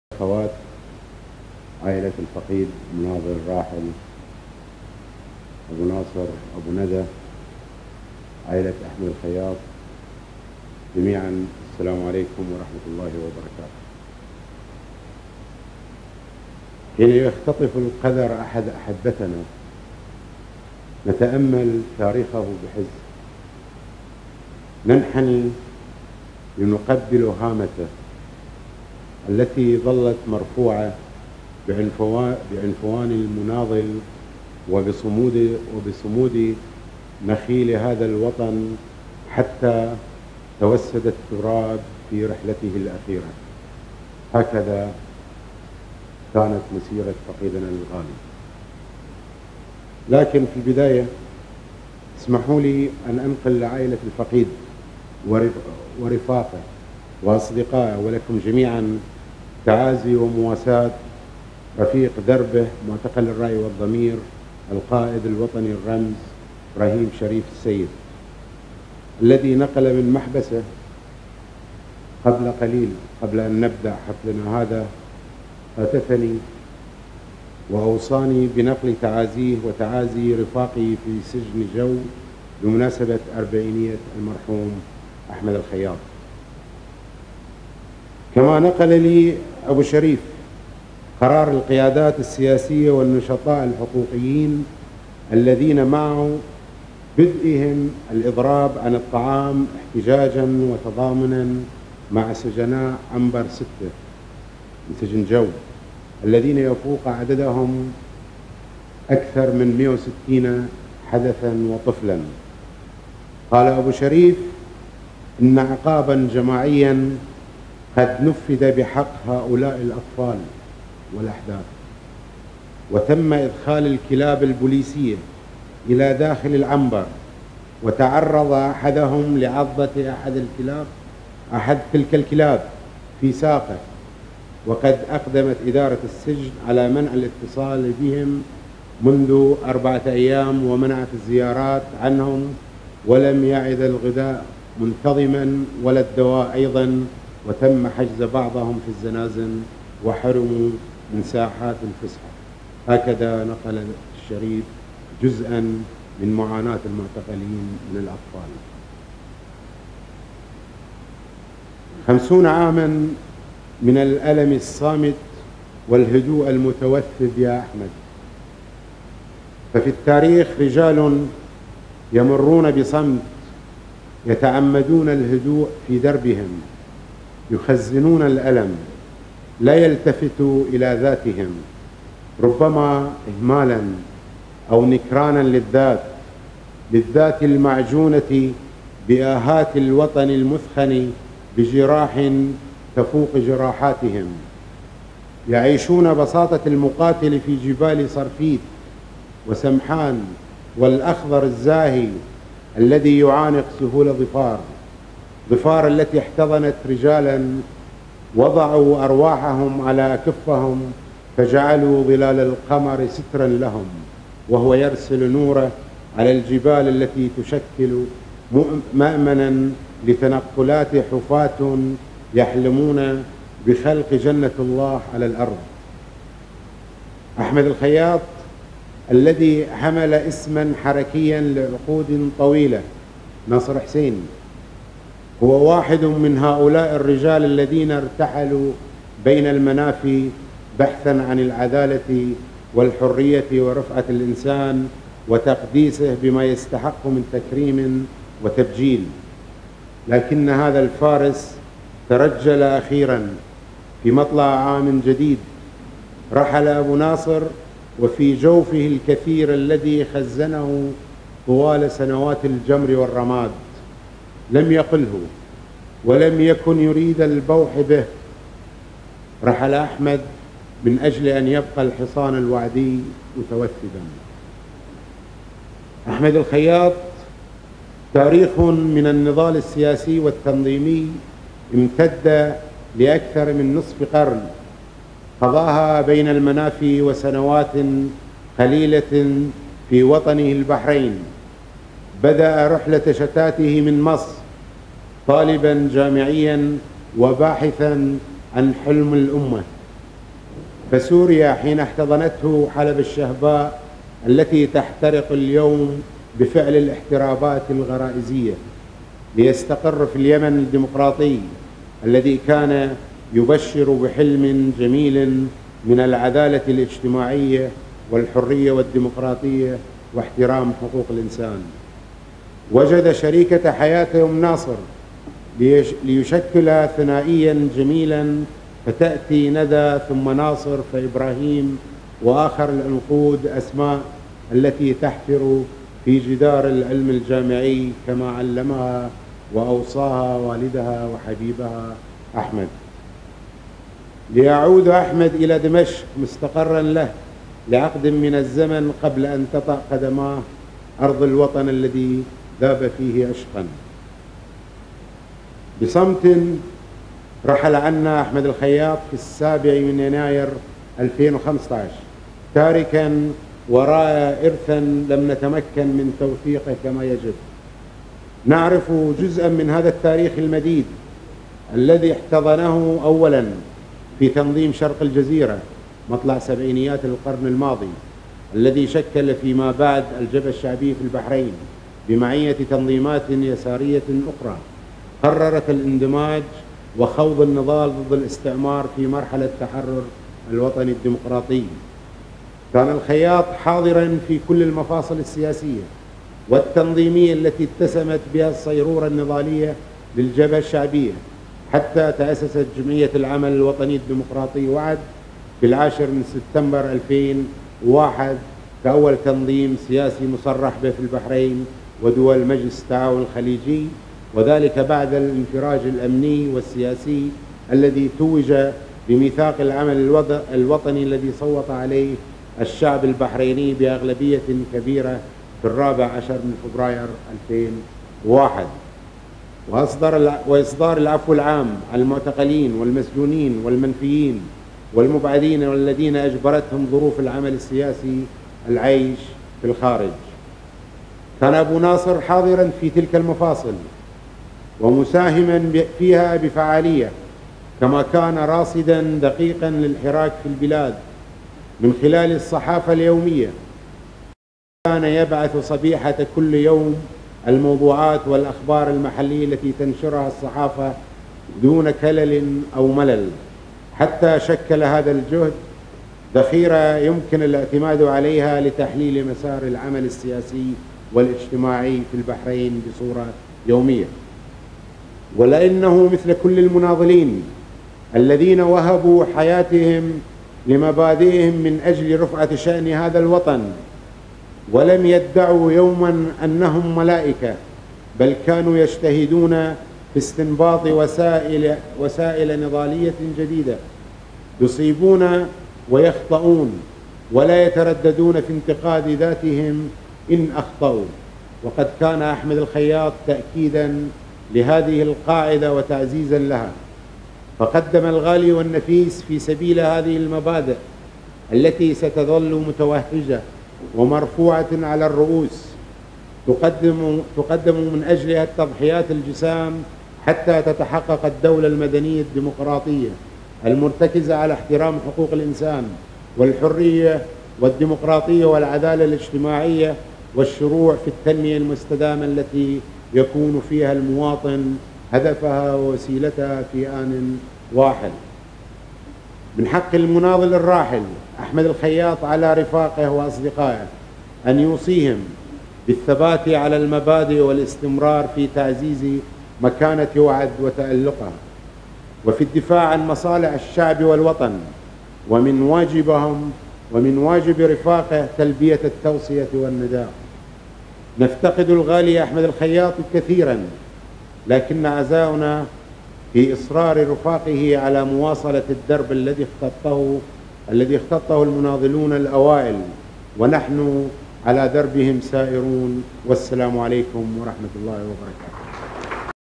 تأبين المناضل أحمد الخياط | كلمة جمعية وعد
تأبين المناضل أحمد الخياط “ترجل صاحب الابتسامة”

18/2/2015